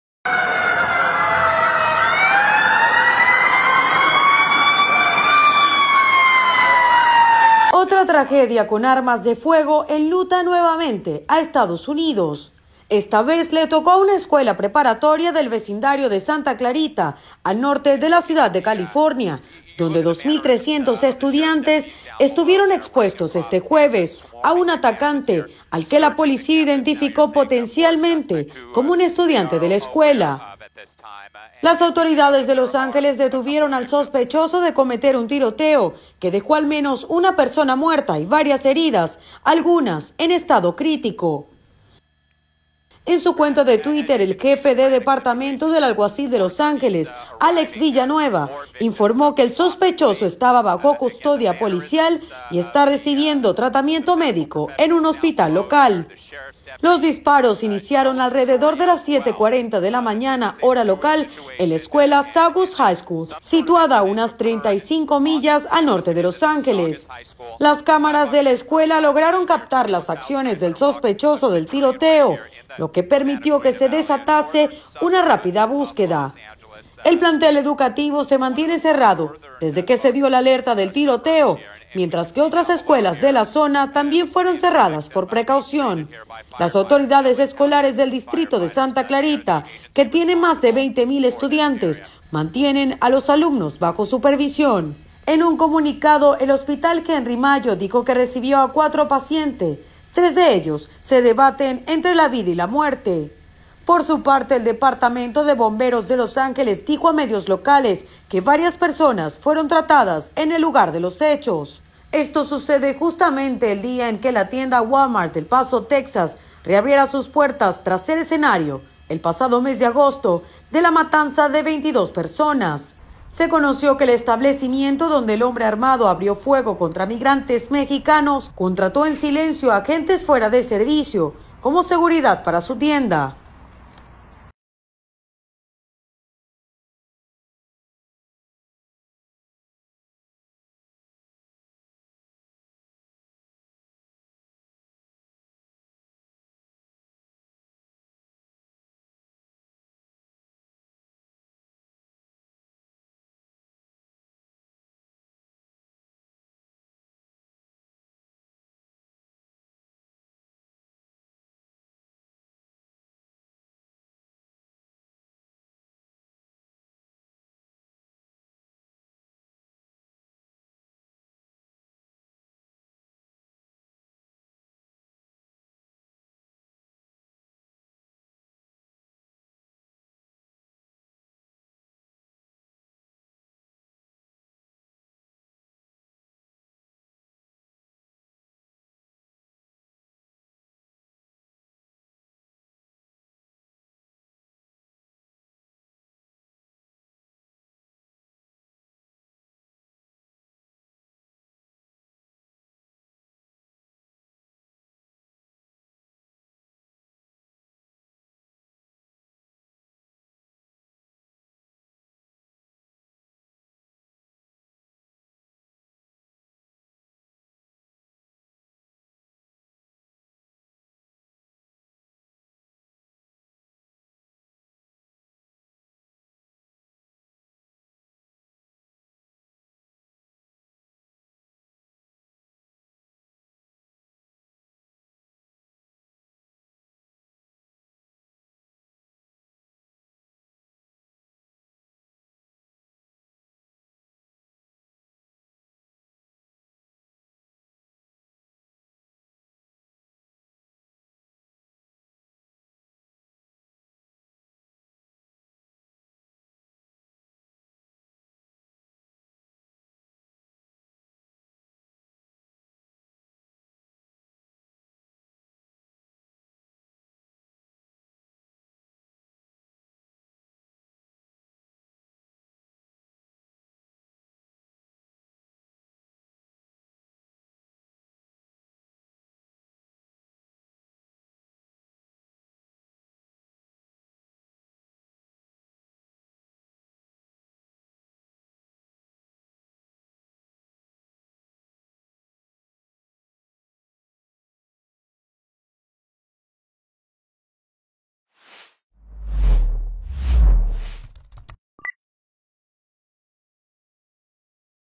Tiroteo en escuela en Los Ángeles dejó una persona muerta y varias heridas
Las autoridades de Los Ángeles detuvieron al sospechoso de cometer un tiroteo que hoy dejó al menos una persona muerta y varias heridas, algunas en estado crítico, en una escuela preparatoria del vecindario de Santa Clarita, al norte de la ciudad californiana de Los Ángeles.

El jefe policial dijo que el sospechoso fue identificado “potencialmente” como un estudiante de la escuela, la escuela, que tiene más de 2.300 estudiantes, se mantiene cerrada desde que se dio la alerta del tiroteo.